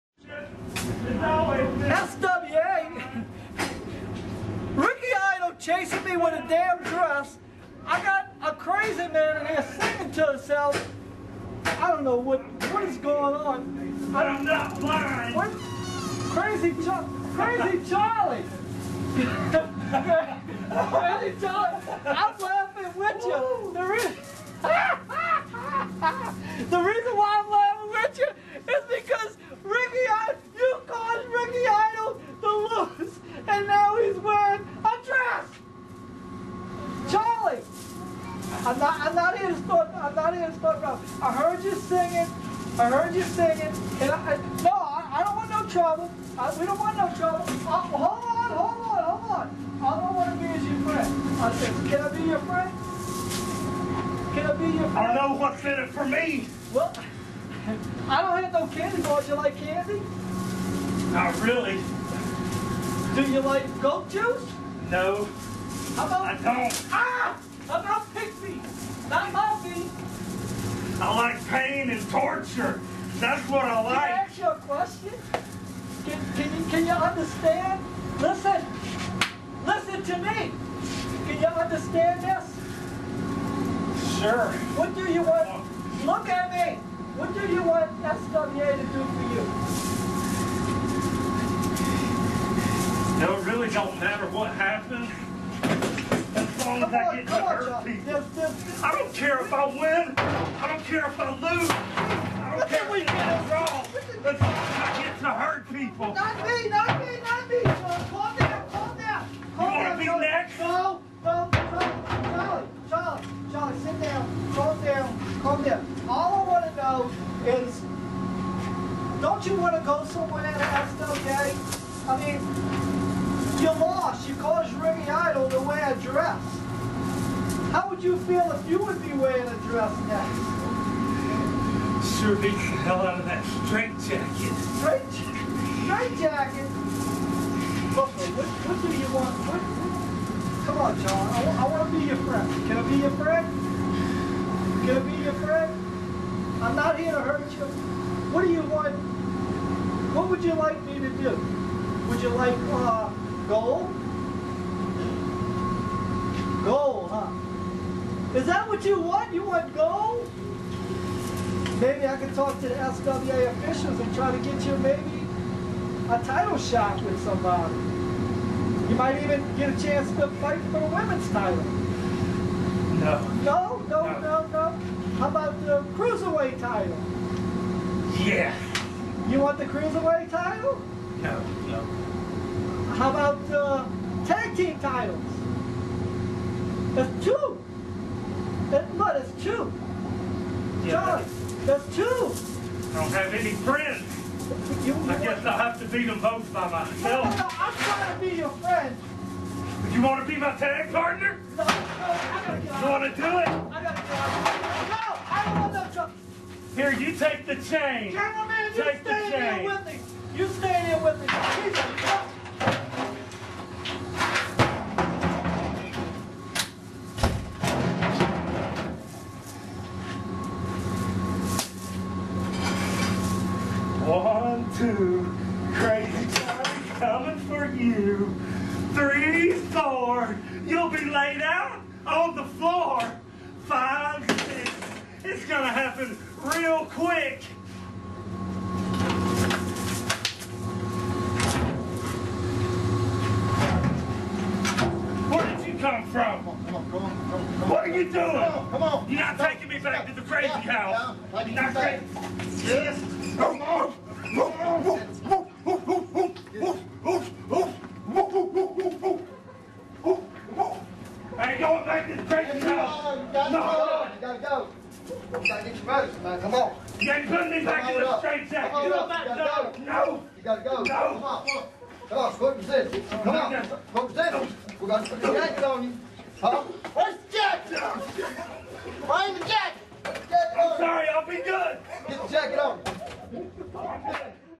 Back to the Crazy House
Al Katz tries to interview Crazy Charlie. Unfortunately he's not willing to cooperate...